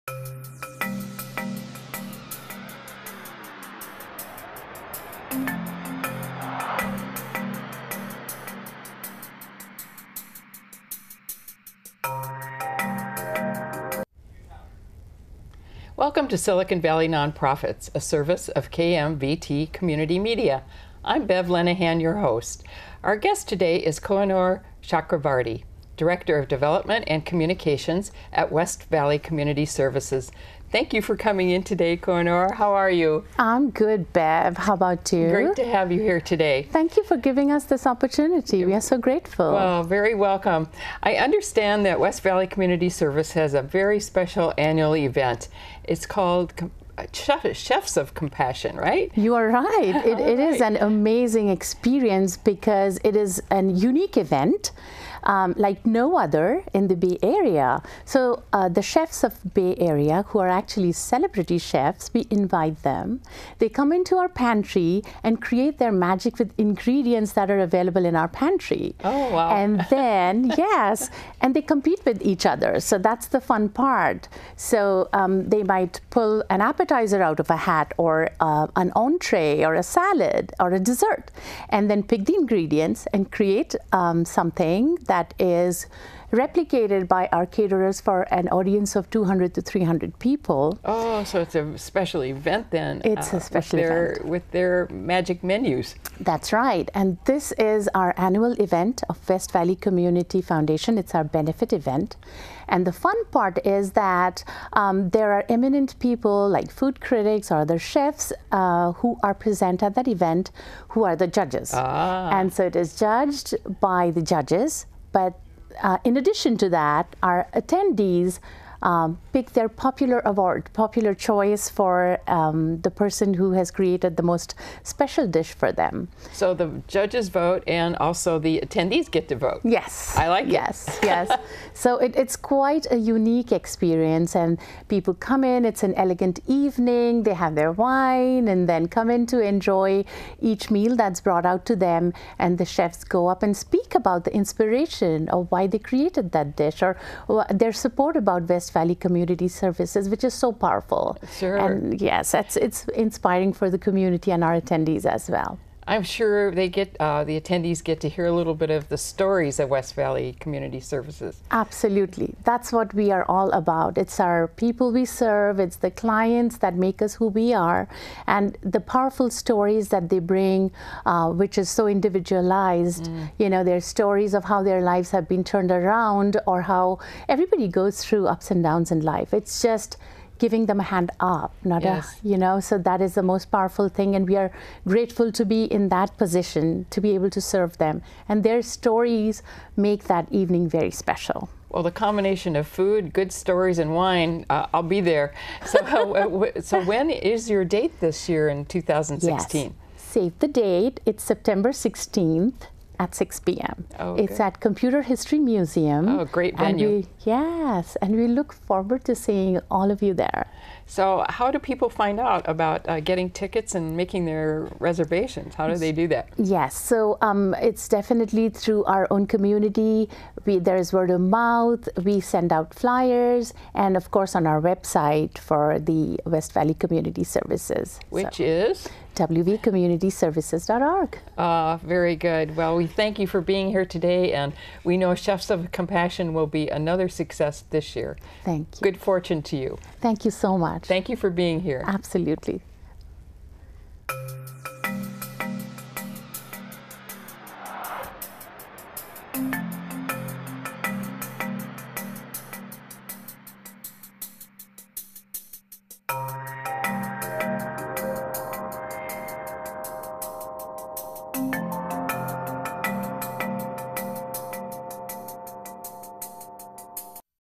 Silicon Valley Nonprofits - Kohinoor Chakravarty, Director of Development and Communications
Kohinoor Chakravarty, Director of Development and Communications. This program was aired on KMVT15 Community Media.